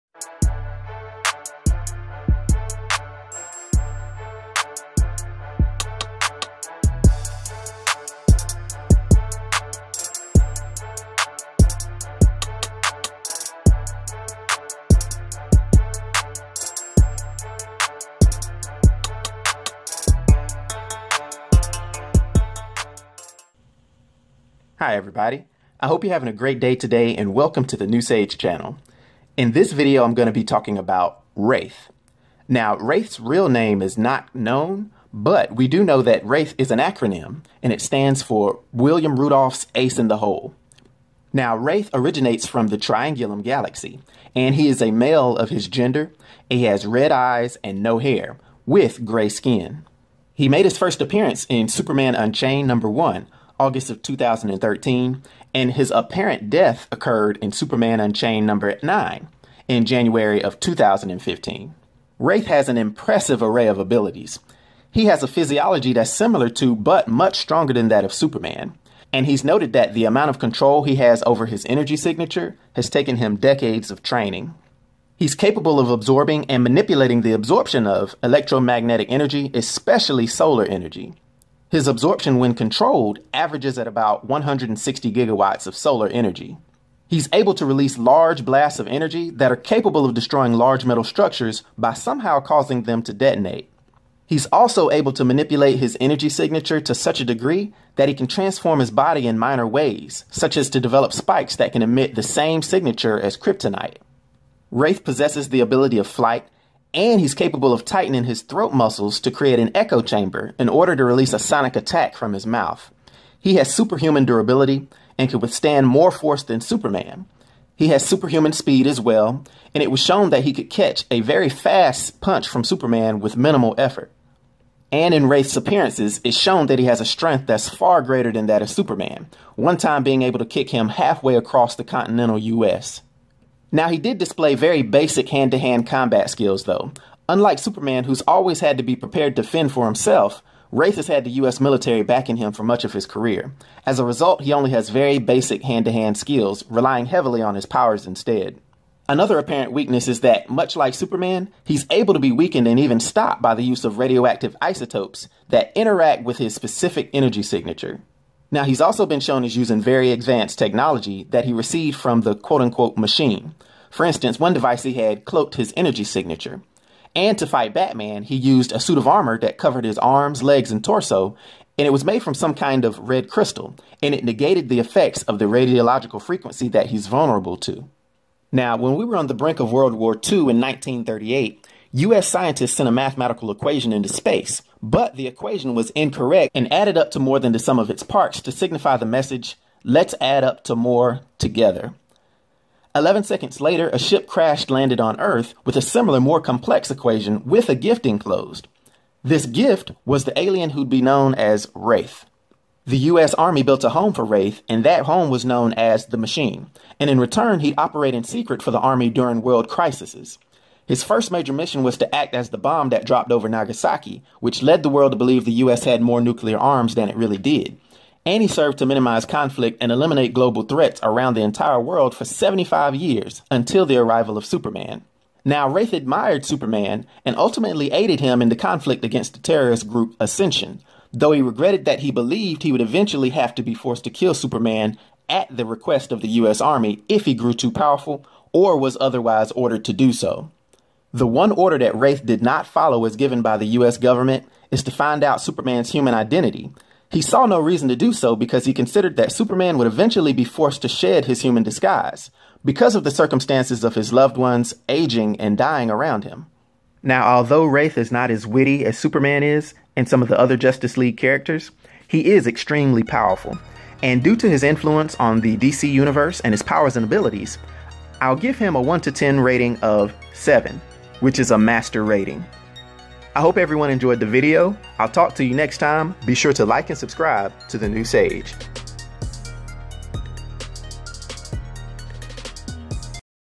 Who is DC Comics Wraith? New "Doomsday," made in U.S.A... Sort of.
This video explores the history, powers, and abilities of the DC Comics character "Wraith." From his arrival to Prime Earth from the Triangulum Galaxy, working for the U.S. Army from "The Machine," helping Superman fight "Ascension," and his massive power giving him an edge over the Man of Steel and the Justice League; Let's take a look at "William Rudolph's Ace in the Hole." Wraith is a powerful new villain that rivals the likes of Doomsday. All including a short, canon biography and my own opinionated "Sage rating."